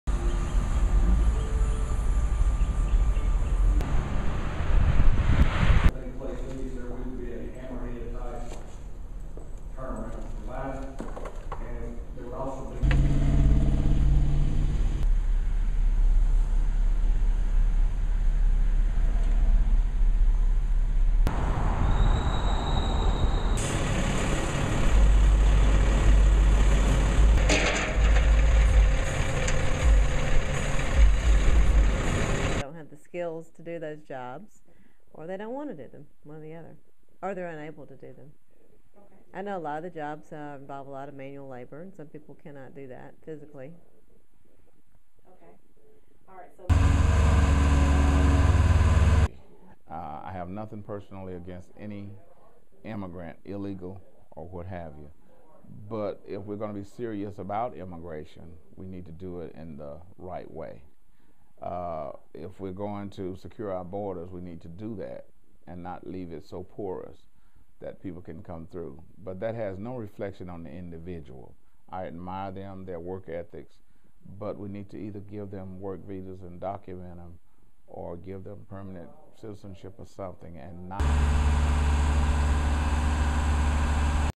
"Re'Chelle Tuner- Alabama new immigration law ruling"
here is a short clip on what Tuscaloosa City Council had to say about the new immigration law.